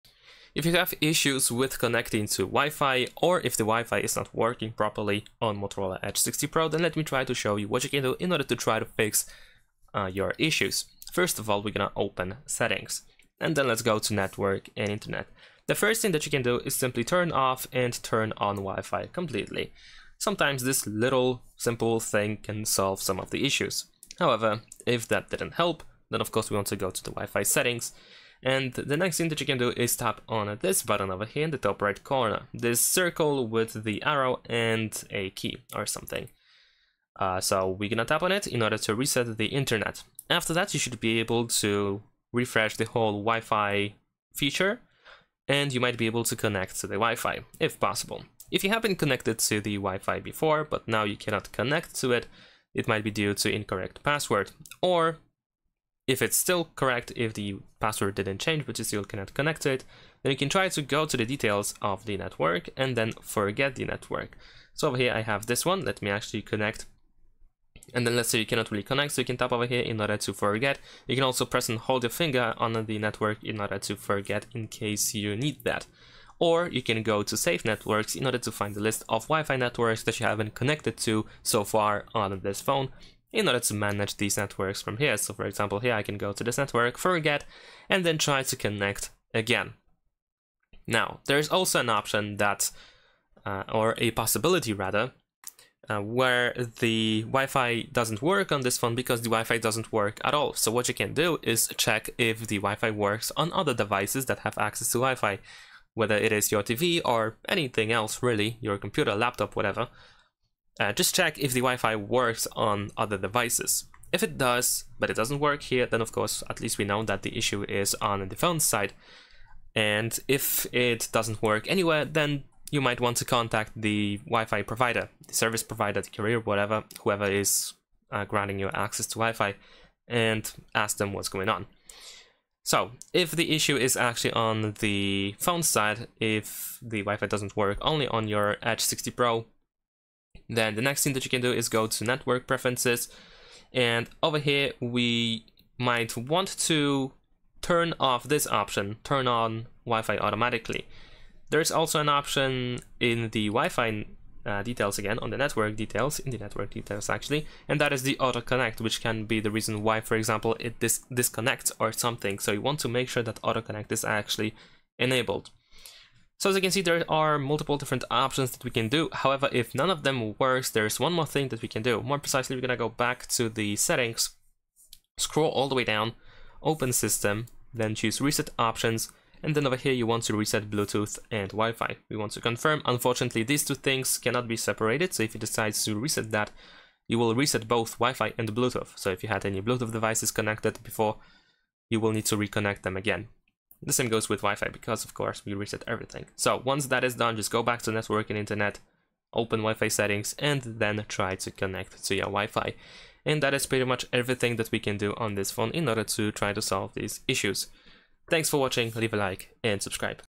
Motorola Edge 60 Pro: How to Fix WiFi Not Connecting (Repair WiFi Issues)
Struggling with WiFi not connecting on your Motorola Edge 60 Pro? This step-by-step guide covers the best troubleshooting fixes to get your WiFi working again. Learn how to reset network settings, forget and reconnect to networks, and solve common connectivity issues on your Motorola Edge 60 Pro. Get back online fast with these proven solutions for WiFi problems on your device.